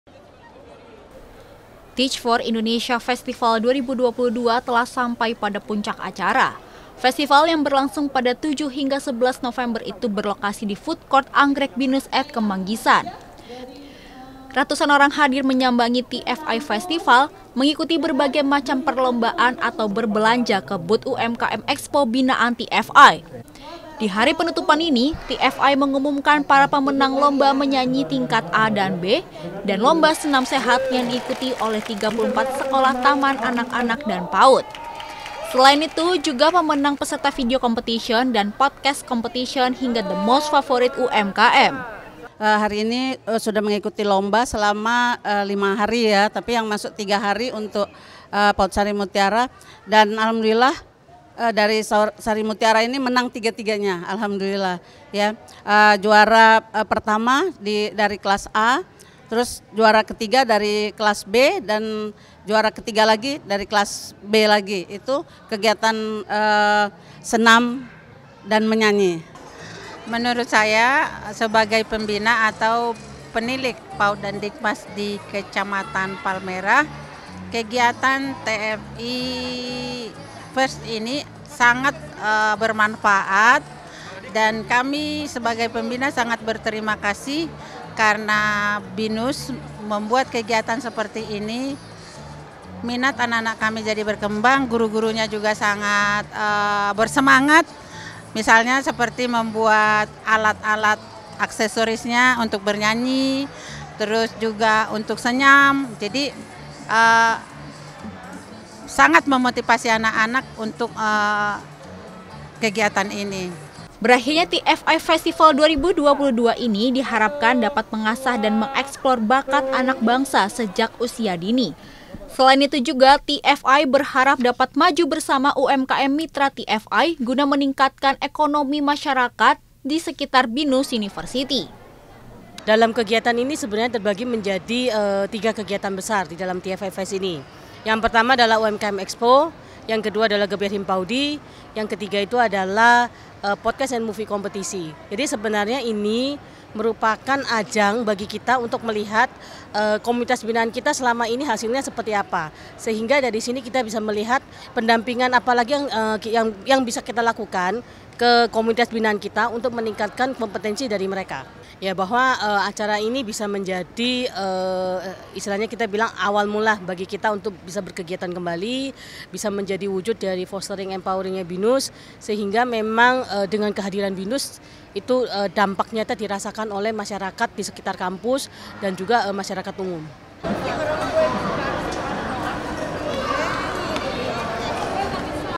Liputan - TFI Festival 2022, Membina Dan Memberdayakan Masyarakat | JURNAL 19
Teach For Indonesia TFI Festival 2022 menutup acaranya dengan serangkaian pengumuman perlombaan. Mulai dari lomba menyanyi hingga favourite UMKM. Festival yang berlangsung selama 4 hari dan dihadiri oleh ratusan orang tersebut sukses digelar.